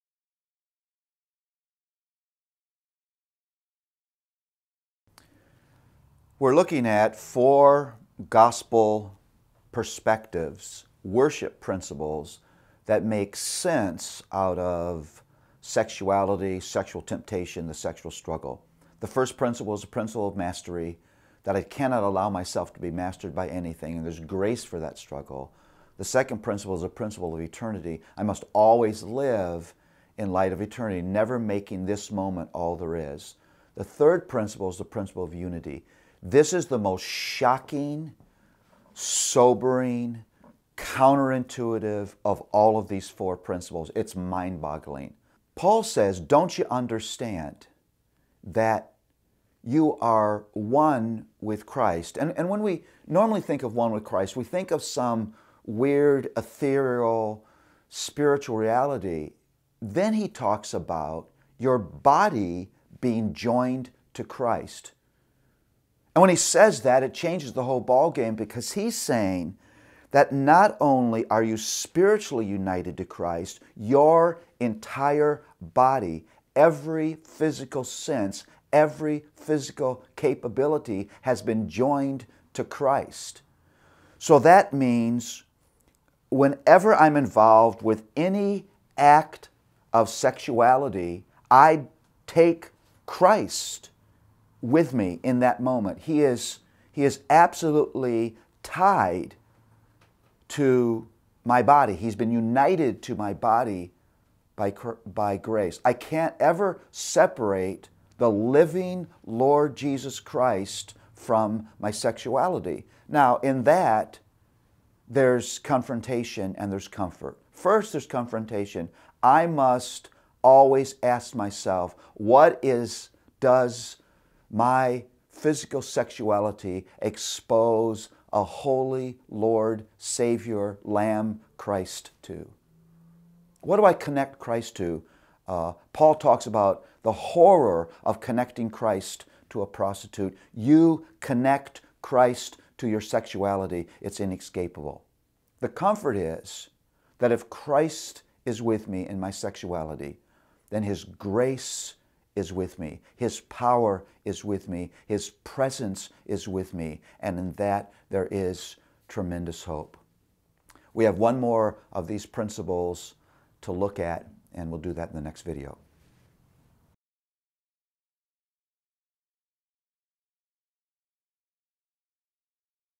The Principle of Unity | Paul David Tripp
In this six-part video series, Paul David Tripp unpacks 1 Corinthians 6, verses 12-20. It's a great diagnostic passage on worship and sexuality, providing four worship principles and two worship commands. In this video, Tripp explains the third principle - the principle of unity.